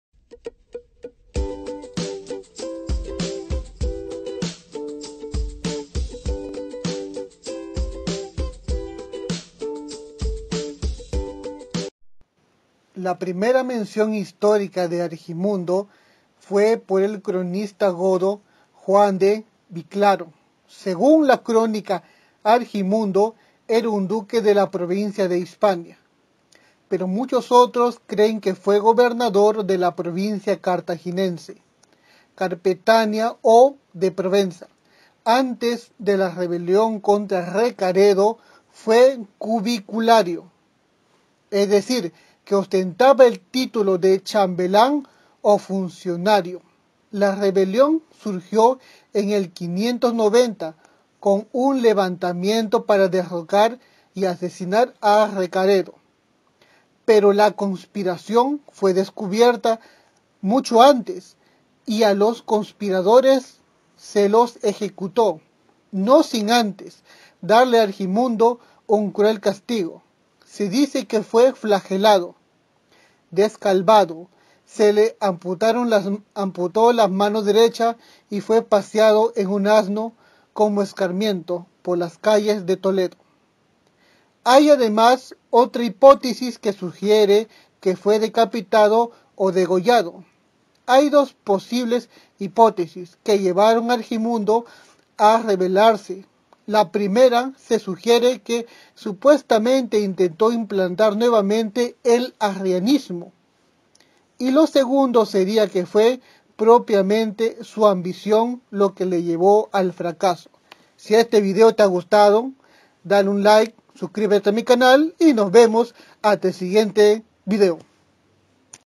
ARGIMUNDO NOBLE HISPANO VISIGODO | BIOGRAFÍA
argimundo se rebelo contra recaredo y fue cruelmente castigado.